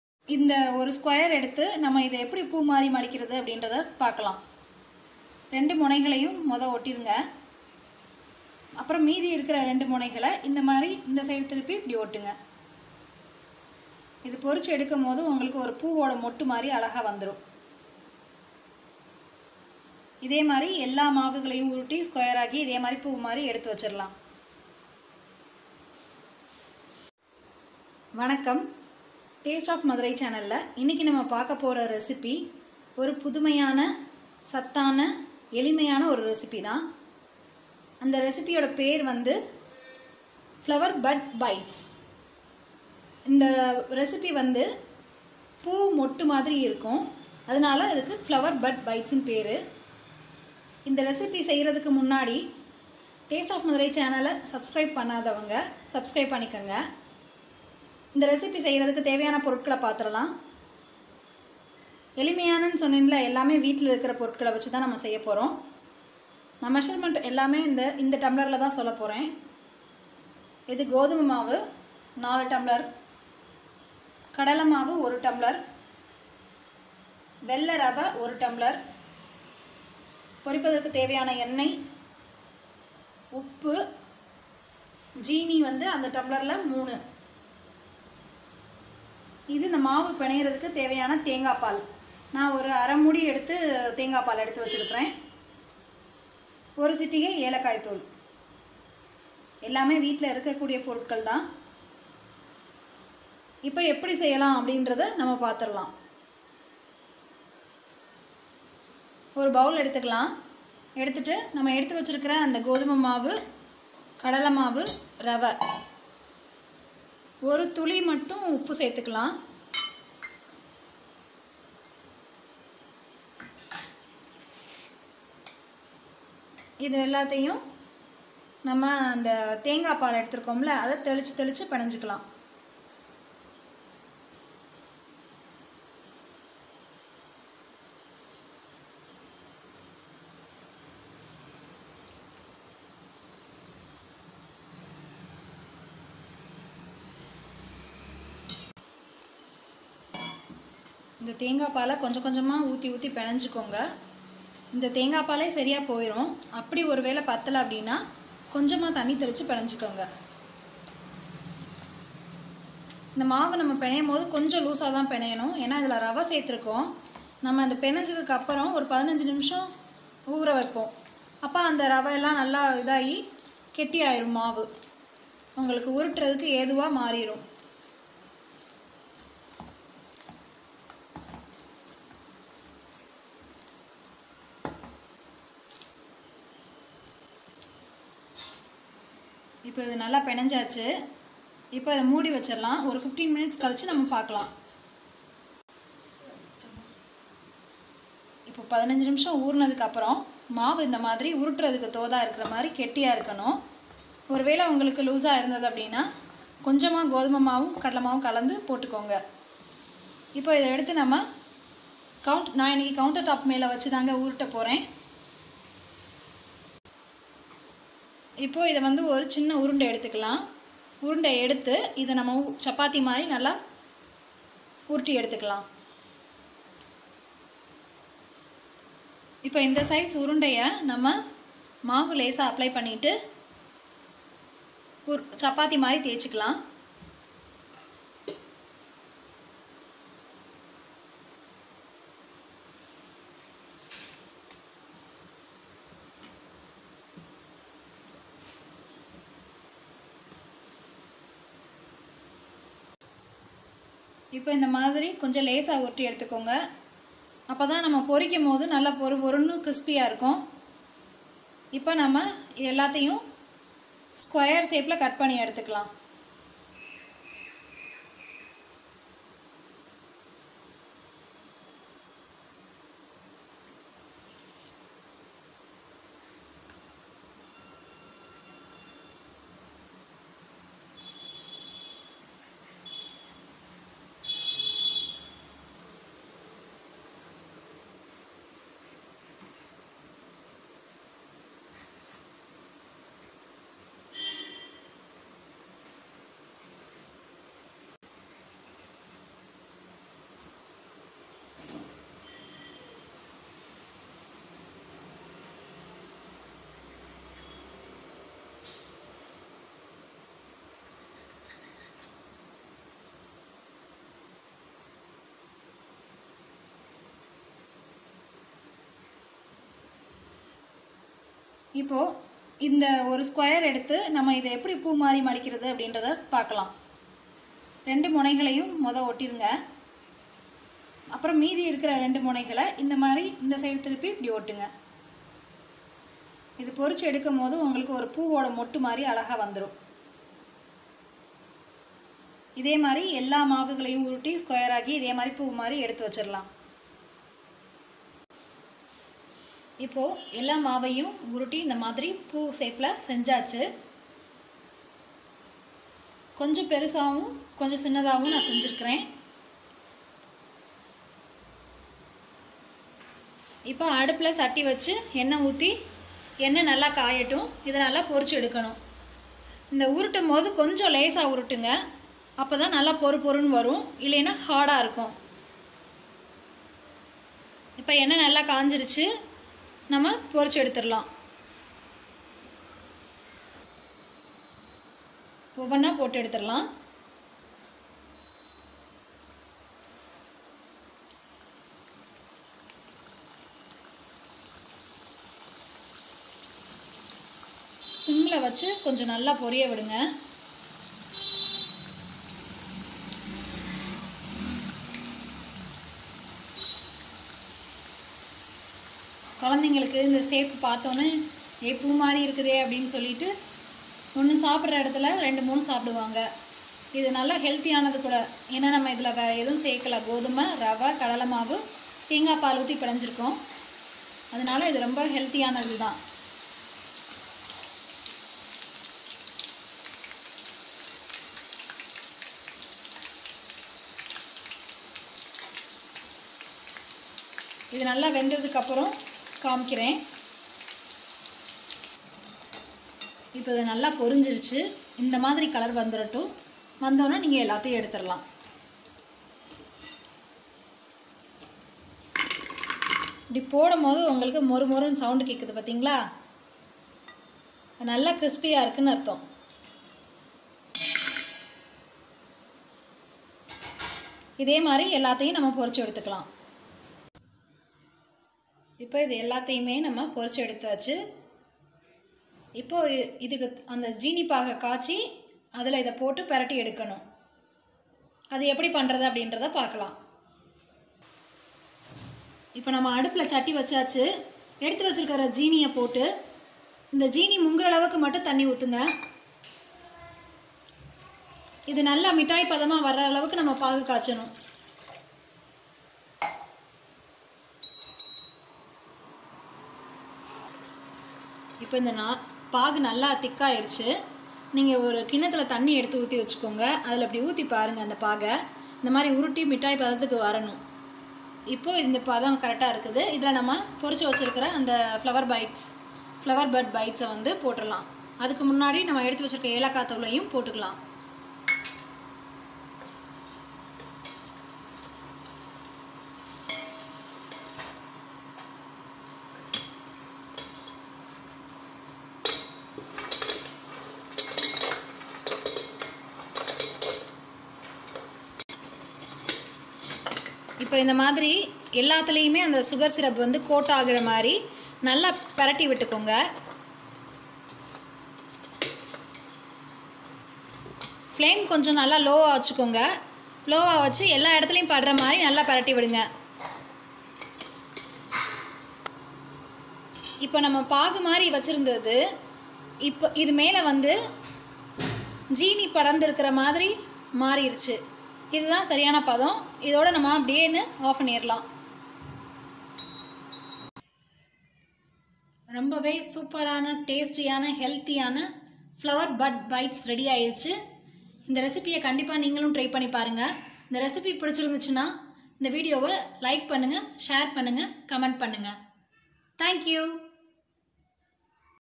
Flower Bud Bite Recipe in Tamil l Sweet Recipe in Tamil l Indian Sweet Recipes in Tamil
In this video, we will see how to make a flower bud bite recipe in Tamil.
Try this video in your home and place your comments below.